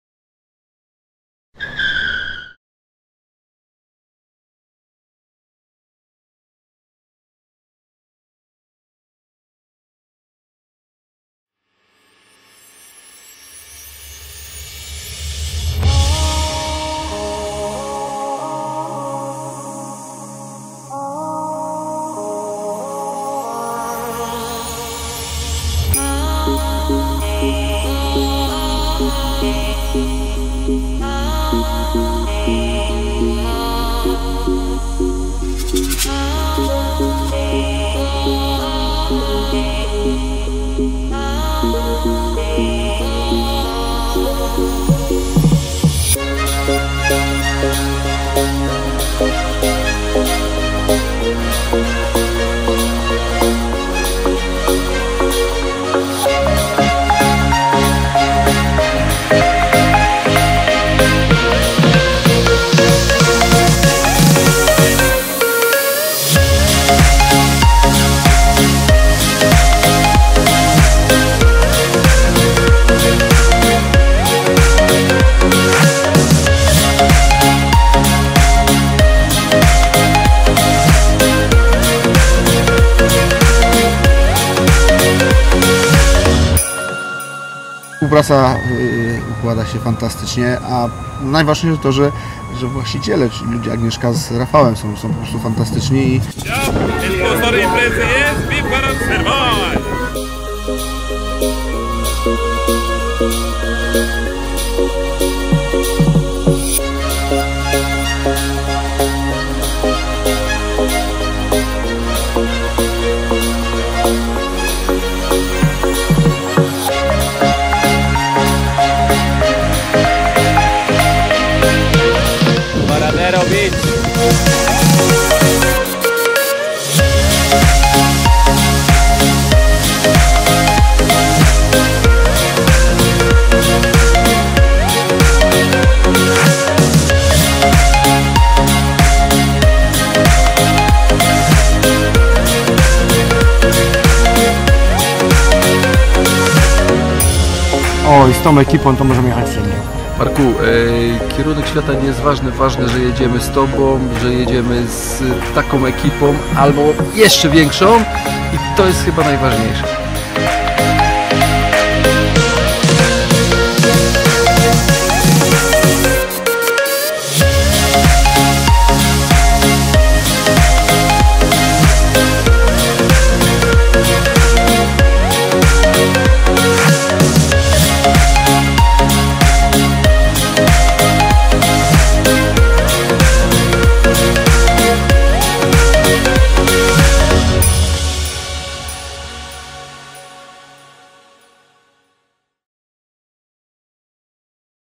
VIP G.S. | CUBA | 29.11-7.12.2017 | TRAVEL TRAILER | MAREK SZAFRAŃSKI CAMERA TRAVEL
Wyjazd gratyfikacyjno - szkoleniowy, dla kontrahentów oraz zwycięzców konkursu sprzedażowego w roku 2017 firmy VIP G.S. Koordynowany oraz filmowany przez Marek Szafrański Camera Travel. Z pozdrowieniami dla całej ekipy!!! :)